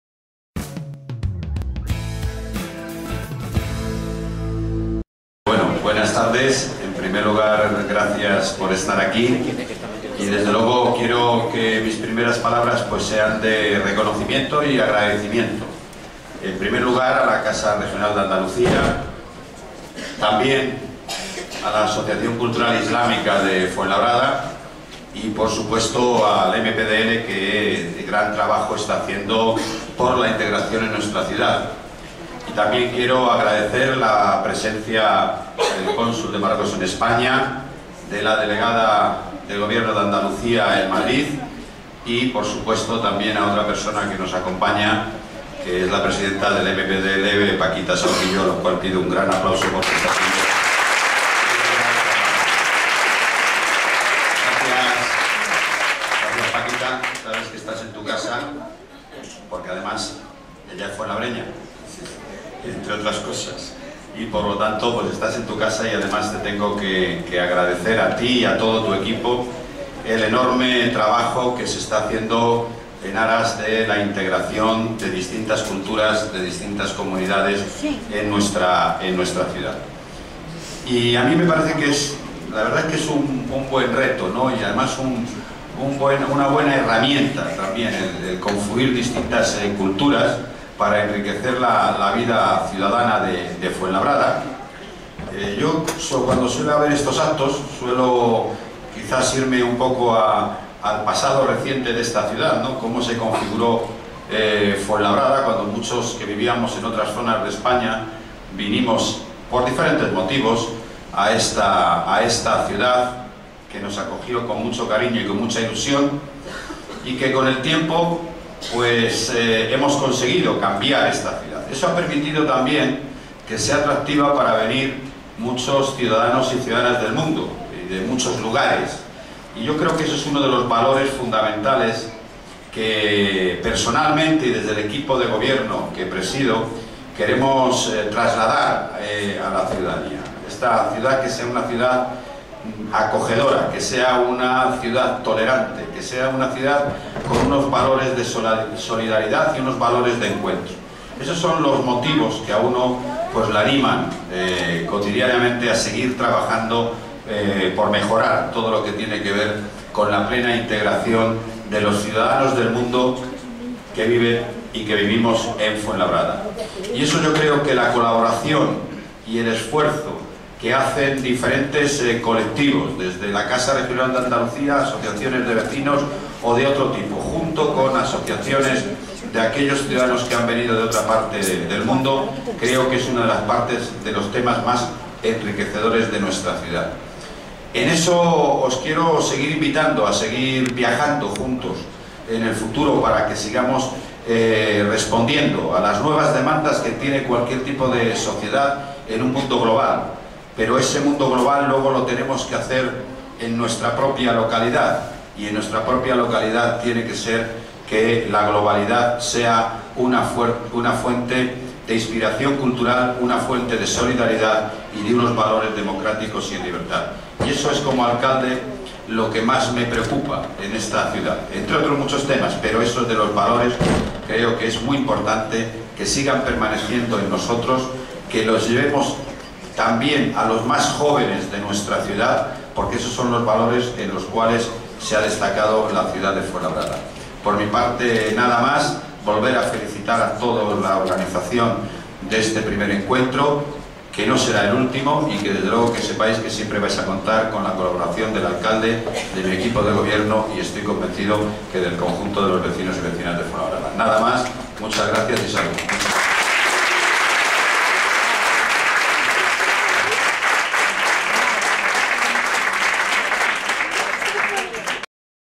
I Encuentro de la Cultura Arábigo - Andalusí en Fuenlabrada
Este sábado 8 de noviembre, se ha celebrado en la Casa Regional Andaluza de Fuenlabrada, el I Encuentro de la Cultura Arábigo - Andalusí. El alcalde Manuel Robles acudió a la inauguración y dirigió unas palabras a l@s asistentes.